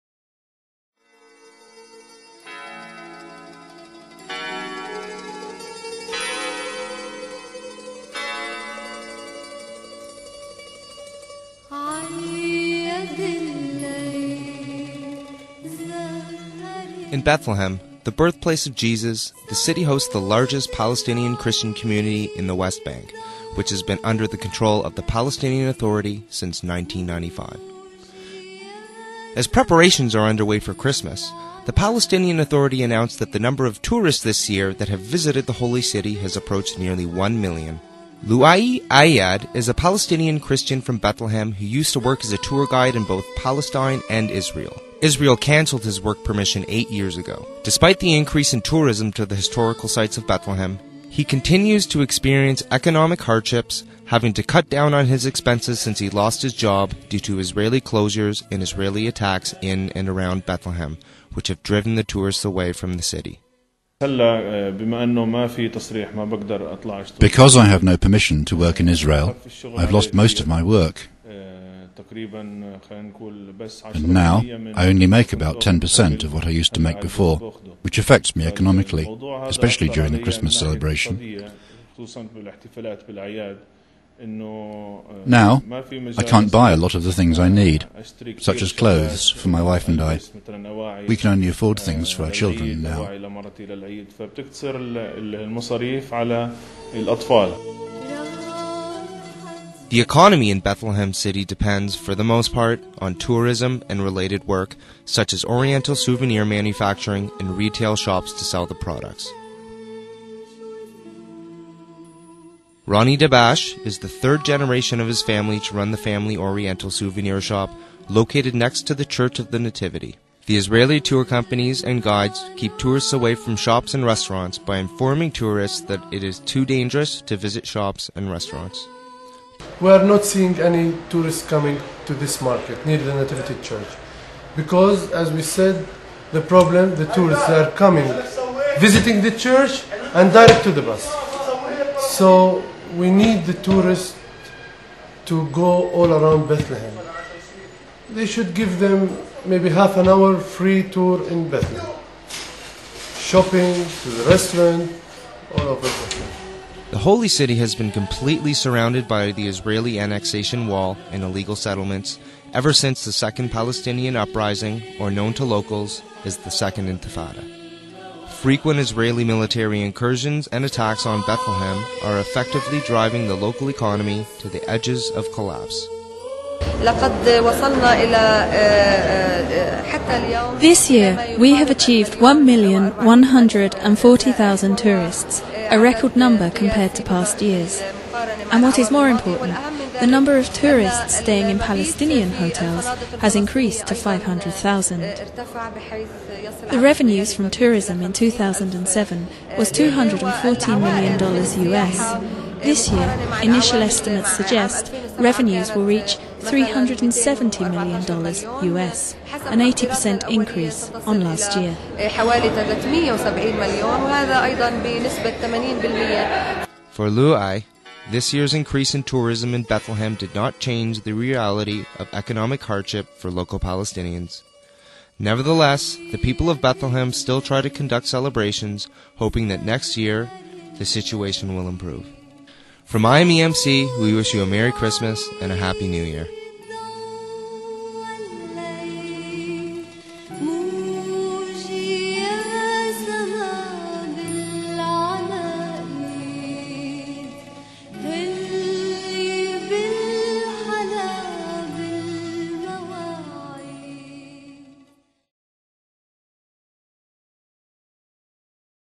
Bethlehem: Christmass 2008
A short film featuring a Palestinian Christian family during Christmas of 2008.  The film is fully produced by IMEMC video Production Unit.

bethlehem, christmas, 2008, nativity, church, Ayyad, Beit Sahour, Christians, Palestine, Palestinian, Israel, wall, occupation, family